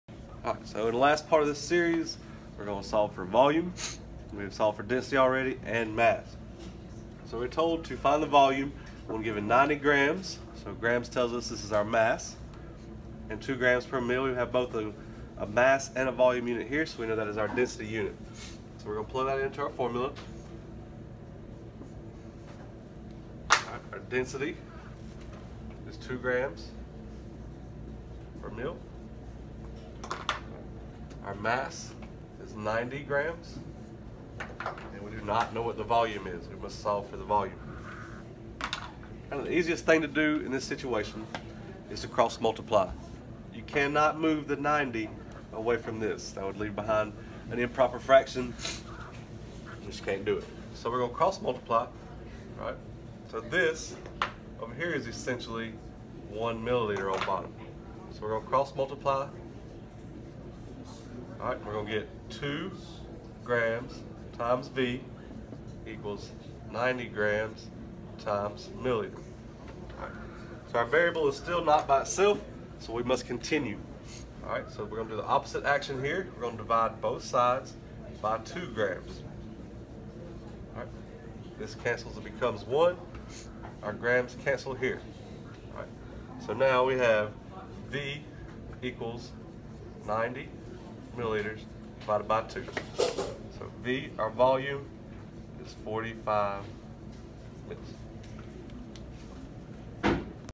How to Solve for Volume using d=m/v
Brief Tutorial of how to solve for volume using the density formula. Video 3 of the density series.